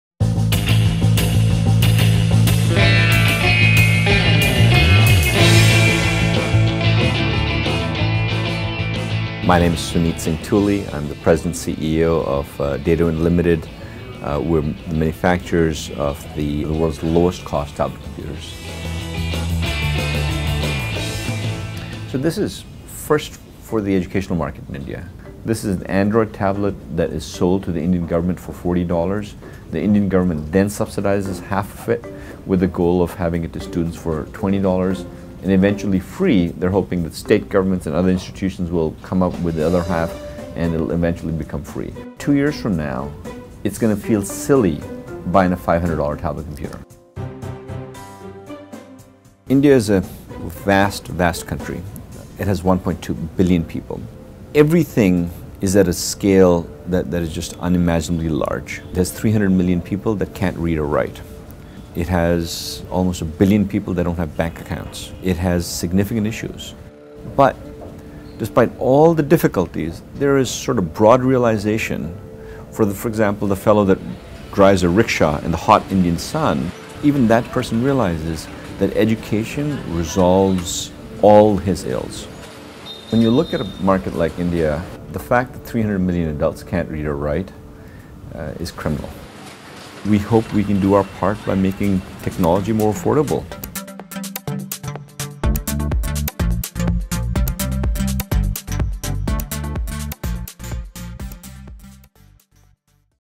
World's Cheapest Tablet
Datawind, a British based company, visited Fast Company. For show-and-tell, Suneet Singh Tuli, Datawind's CEO, brought his low cost tablet, which he hopes to sell to the Indian government for as little as $40. The 7" device boasts a touch screen, an Android operating system, WiFi and a 1.0 GHz processor.

Video by Vivek Kemp